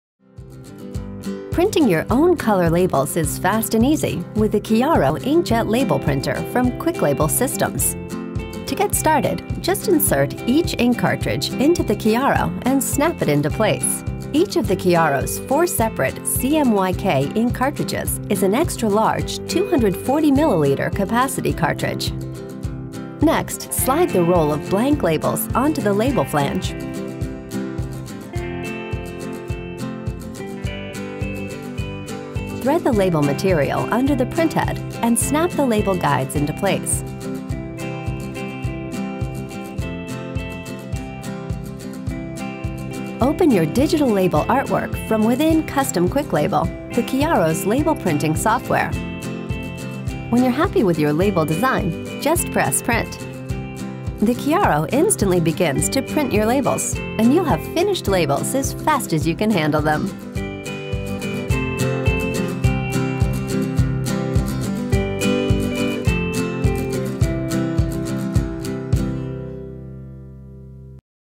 How to Start Printing Your Own Labels with the Kiaro! Inkjet Label Printer
Watch how to print your own labels with the Kiaro! inkjet label printer. The Kiaro! is a ground-breaking color inkjet label printer. Where the Kiaro! stands apart from other color inkjet label printers is that it doesn't require a speed/quality trade-off. The Kiaro! prints color labels in high resolution faster than any other tabletop inkjet label printer. Even at its 8 ips speed (40 feet per minute), you will get great looking, vibrant, color labels in outstanding 1200 dpi resolution. With four separate, large-capacity ink cartridges and low running costs, the Kiaro! label printer is a perfect fit for any packaging room or production line operation.